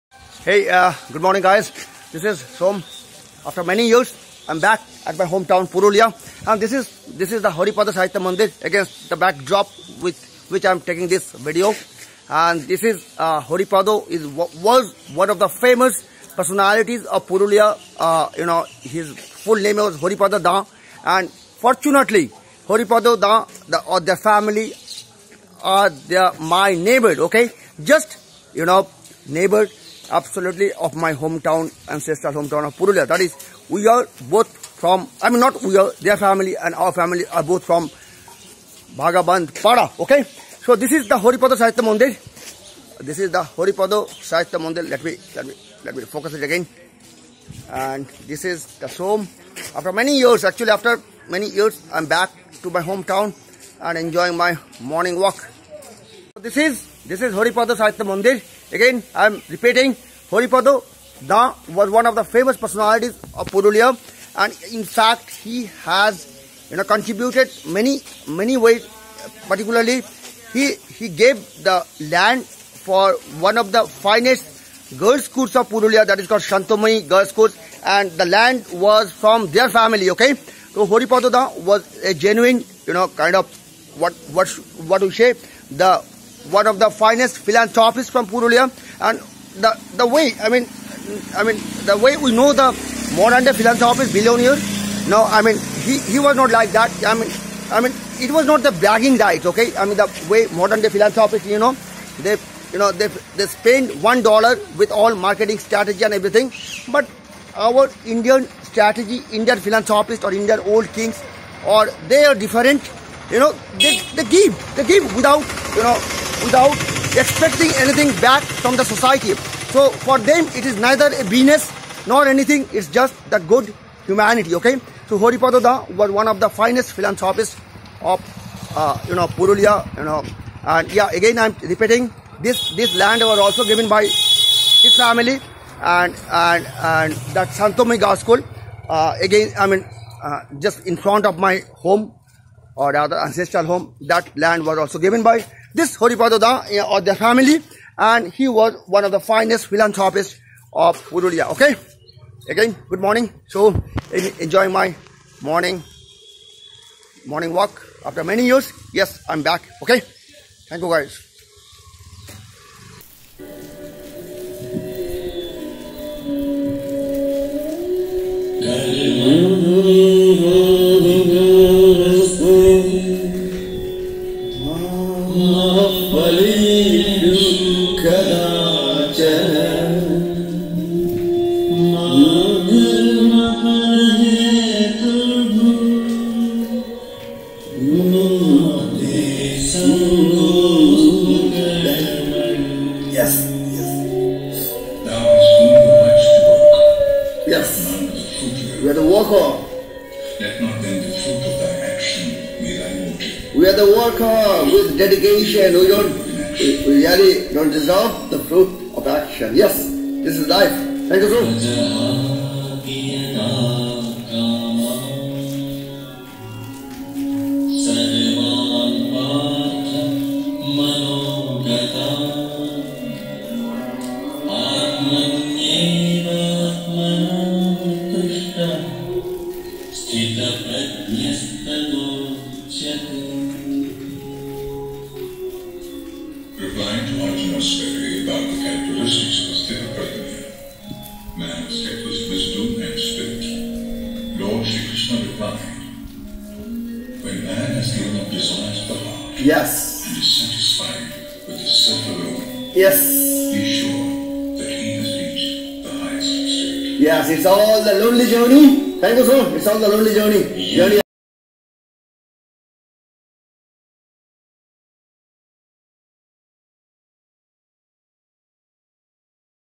haripada dan - one of the early days philanthropists of purulia
in Bharat - the act of philanthropy is done with the true sense of humanity - without any hidden agenda attached to it - as practised by many modern day billionaire American and European philanthropists - who usually practise philanthropy with a hidden business agenda